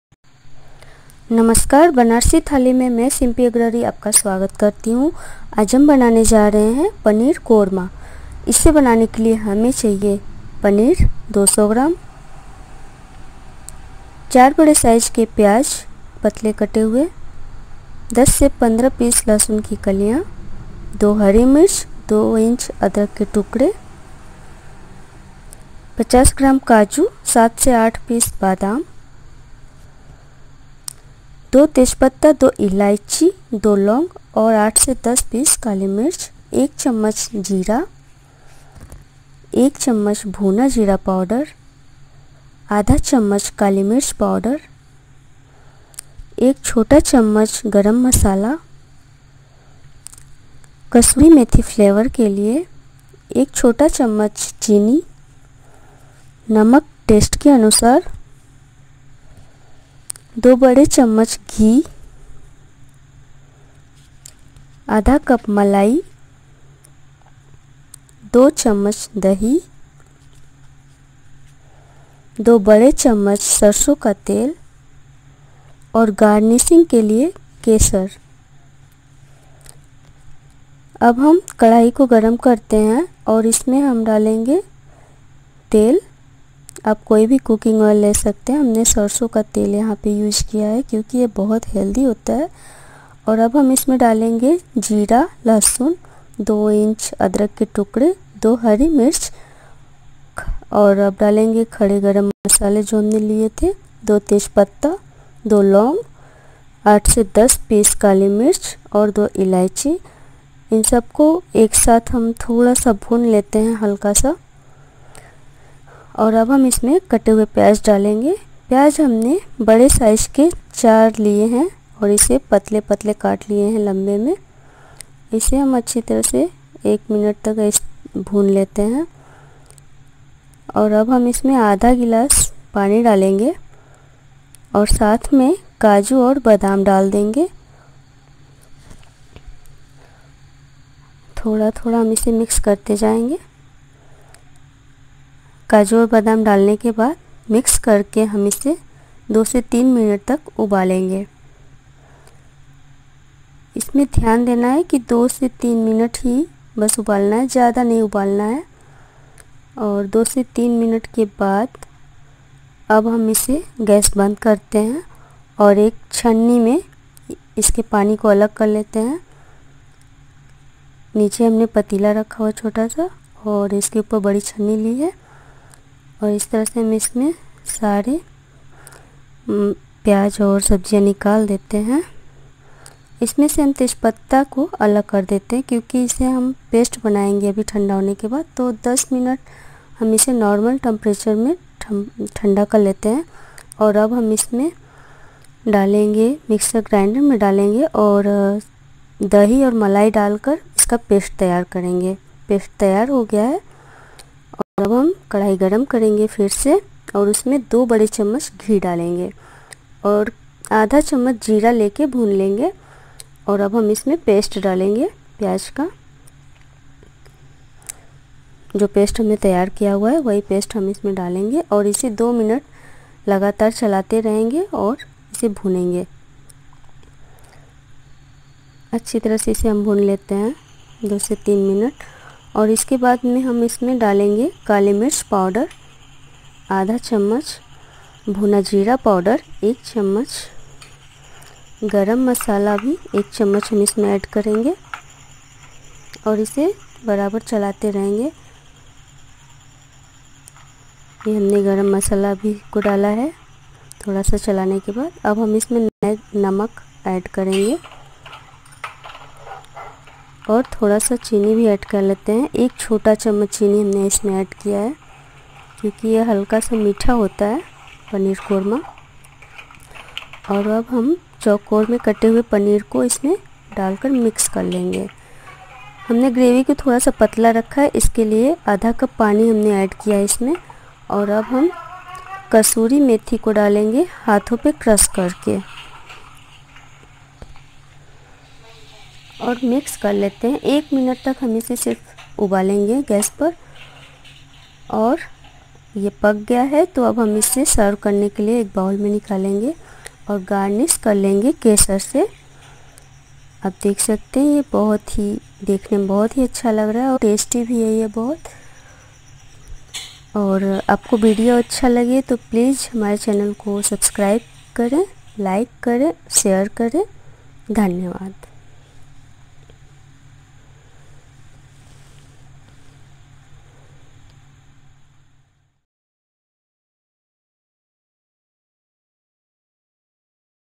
Paneer Malai Korma || By Simpi Agrahari
सामग्री-
पनीर-200 g.
प्याज- 4
लहसुन की कलियां-10-15
अदरक एक इंच
हरा मिर्च- 2
काजू 10-15
बादाम 4-5
काली मिर्च एक छोटी चम्मच
तेजपत्ता- 2
इलायची- 2
 दालचीनी पाउडर एक चुटकी
जीरा एक छोटी चम्मच
घी 2 बड़े चम्मच
मलाई 1/2कप
नमक स्वाानुसार
 चीनी दो चुटकी
नींबू का रस एक चम्मच
विधि-
एक बड़ा चम्मच तेल गरम करें उसमें जीरा, हरी मिर्च,तेजपत्ता, इलायची, काली मिर्च, काजू, बादाम, कटे हुए प्याज,लहसुन, अदरक को दो मिनट तक भूने फिर उसमें एक गिलास पानी डालकर दो से तीन मिनट तक उबालें। अब इसे एक छन्नी से छाने ताकी उसका पानी निकल जाएं और उसमें से तेजपत्ता को अलग कर लें और सारी सामग्री को ठंडा करके मिक्सर ग्राइंडर में मलाई थोड़ा सा दूध लेकर पीस लें।अब कढ़ाई को गरम करे उसमें एक बड़ा चम्मच घी डाले और पेस्ट को दो से तीन मिनट तक भूनें फिर इसमें आधा चम्मच गरम मसाला , पनीर को डालकर अच्छी तरह मिक्स करें अब उसमें आधा कप पानी डालकर चलाएं दो से तीन मिनट पकाएं अब उसमें काली मिर्च पाउडर, दालचीनी पाउडर, कसूरी मेथी,नमक स्वदानुसार दो चुटकी चीनी डाले और एक चम्मच नींबू का रस डालकर अच्छी तरह से मिलाकर गैस बन्द करें। और रोटी या पराठा के साथ सर्व करें।